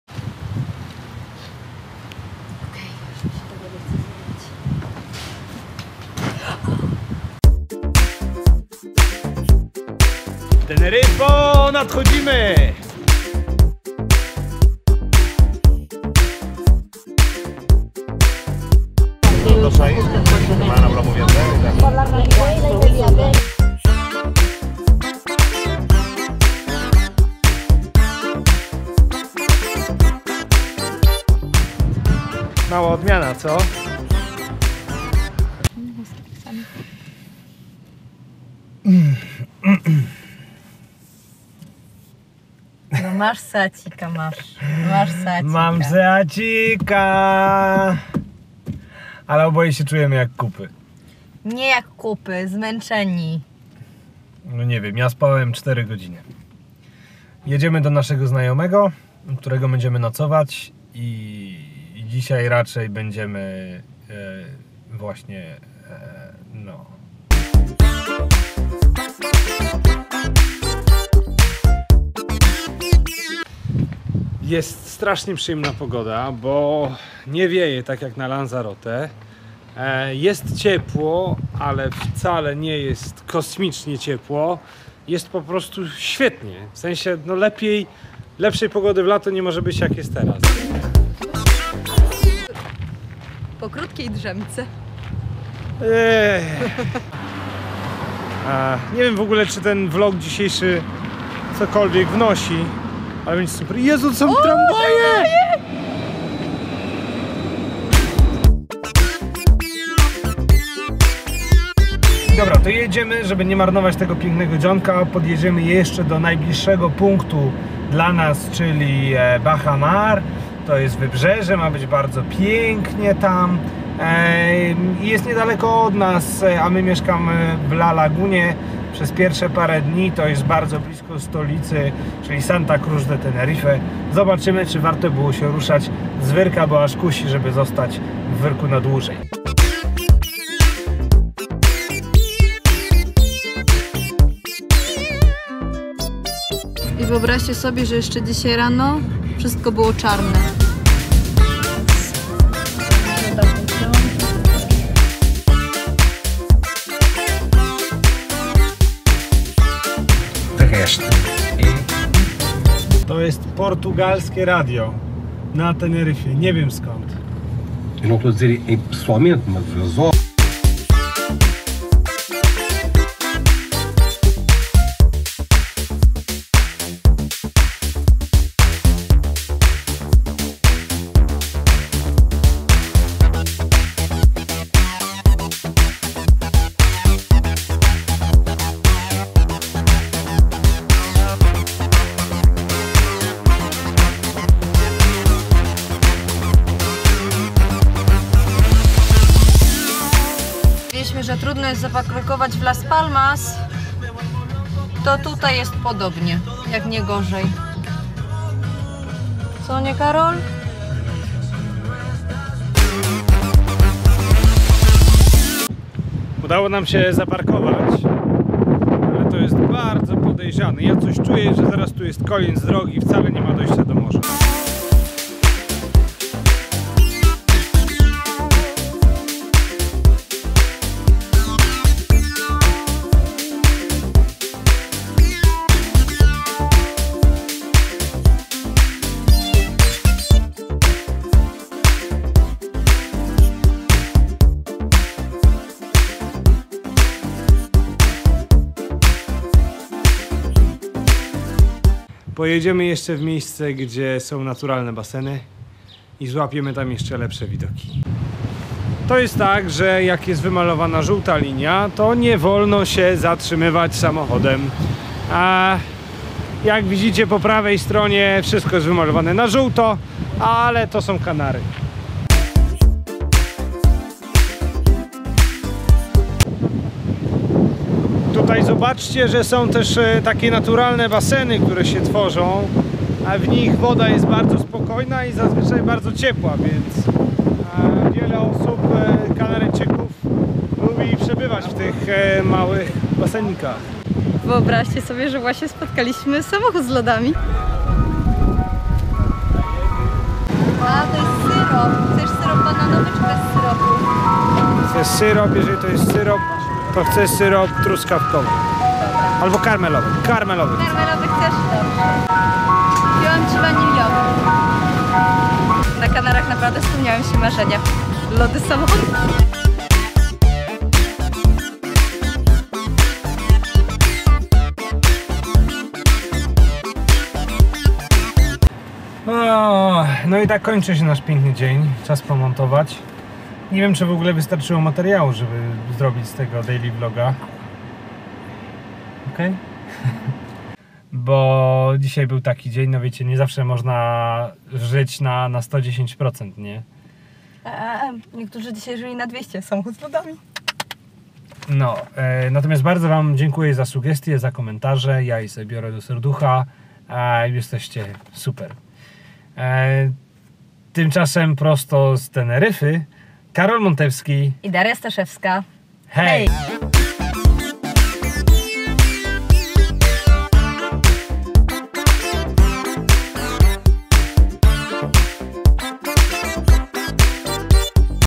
Teneryfo, nadchodzimy! || VlogzKanarów #5
Po przepięknej i spokojnej Lanzarote czas udać się na bardziej żywą i zieloną Teneryfę! Co prawda forma nie była na 100% po krótkiej nocy, ale i tak udało nam się zwiedzić południowe wybrzeże i miejscowość Bajamar. A Daria zjadła swojego pierwszego loda z grającej ciężarówki!

Podoba się? Daj lajka, suba albo udostępnij!